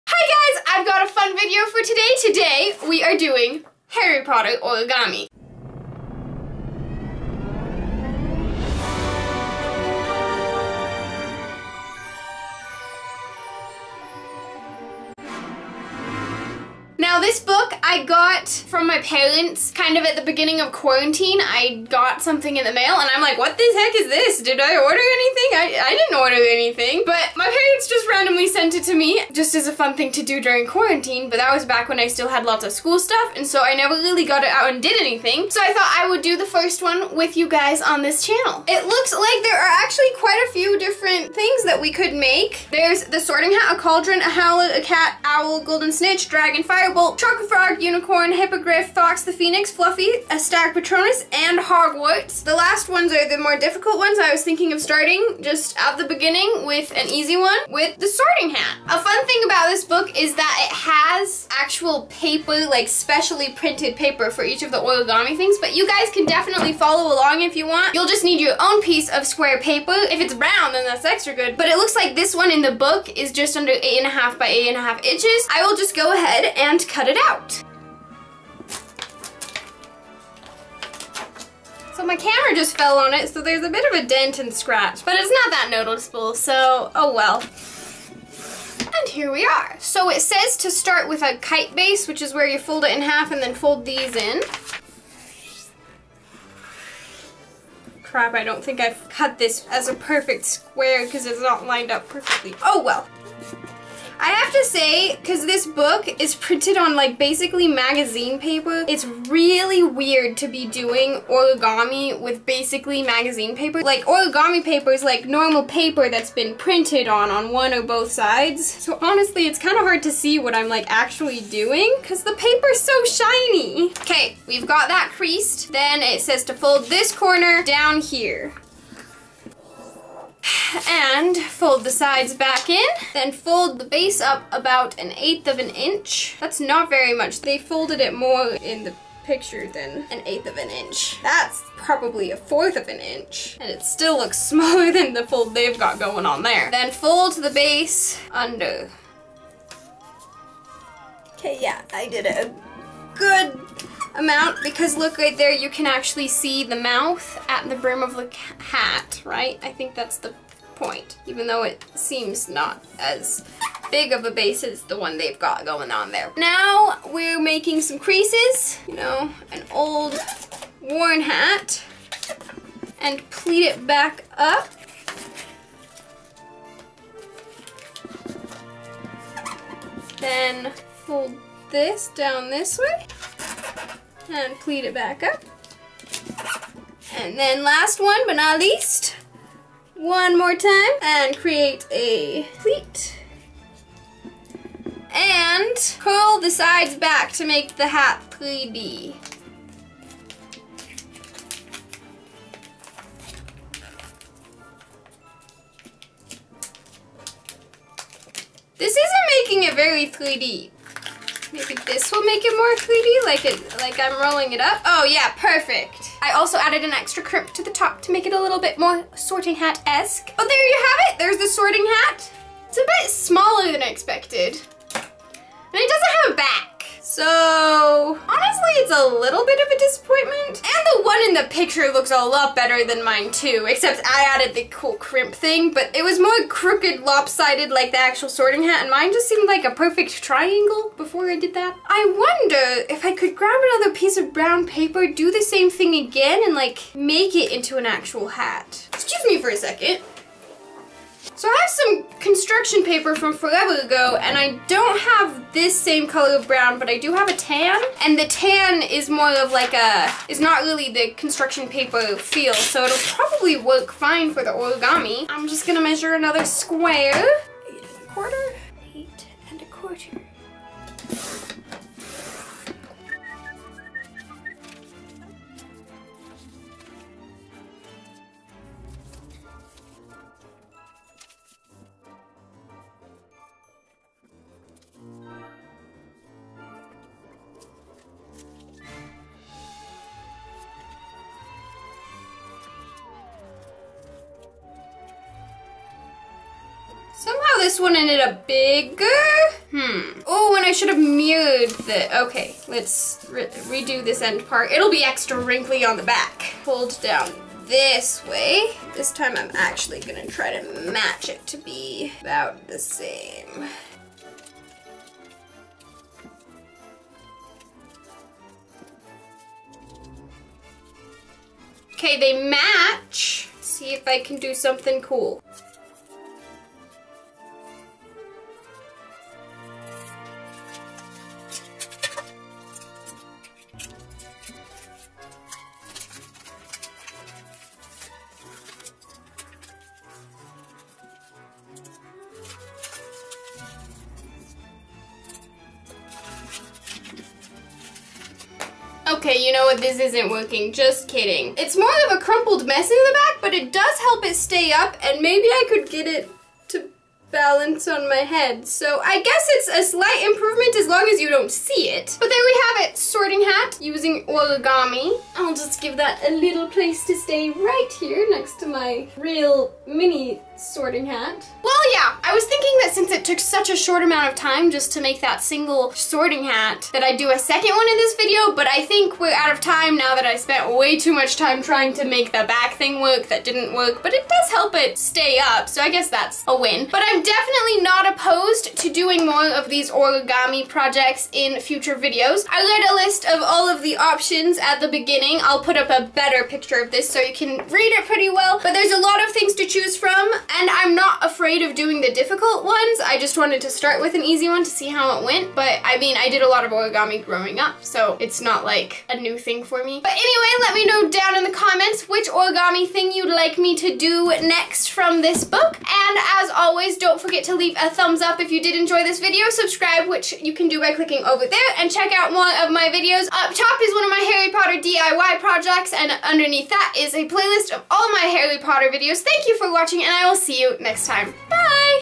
Harry Potter Origami: The Sorting Hat
In this video, I try making an origami sorting hat from a book I have and then try improving it a little.